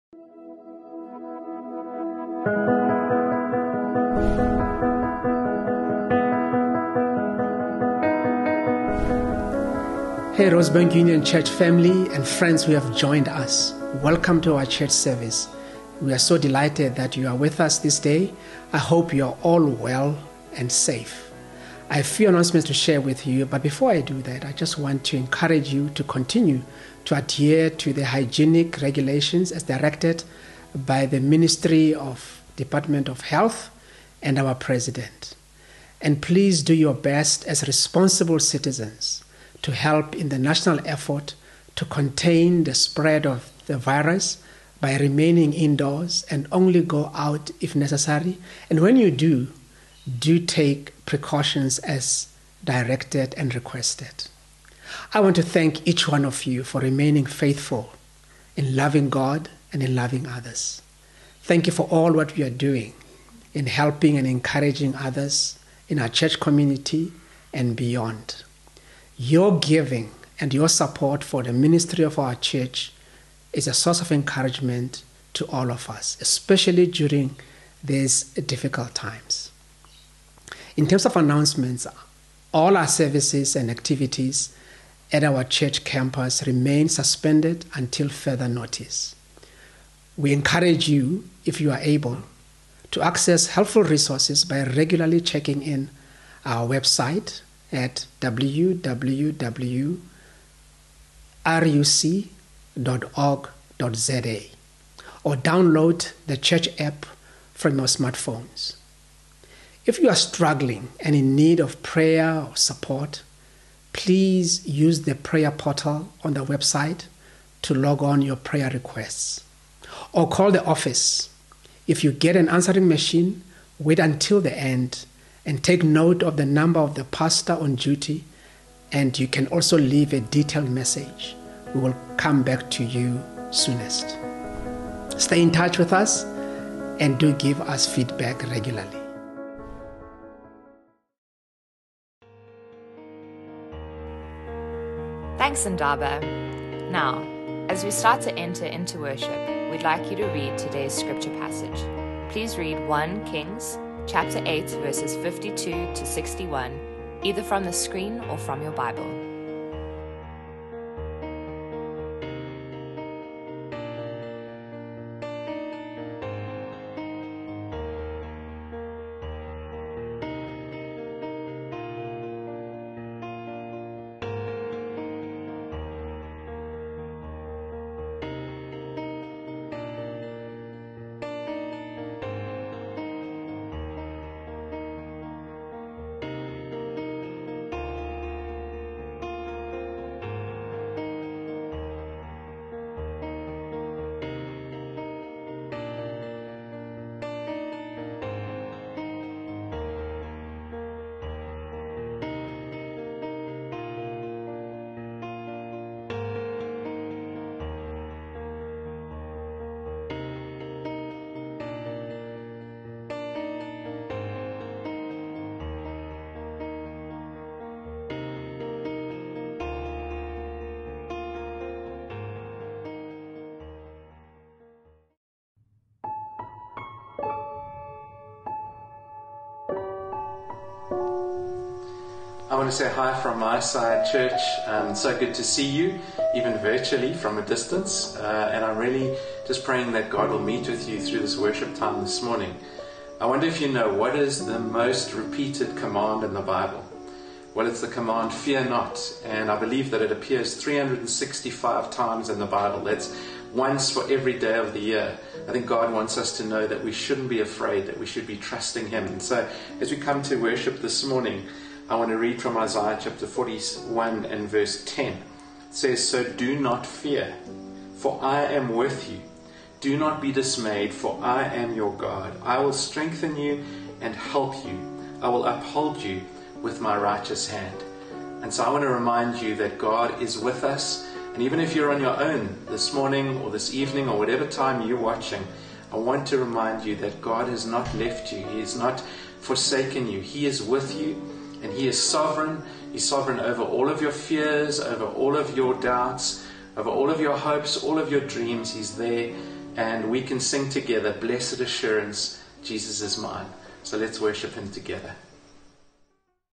20200405 Welcome and Call to Worship
Welcome, Announcements, Scripture Reading and the Call to Worship.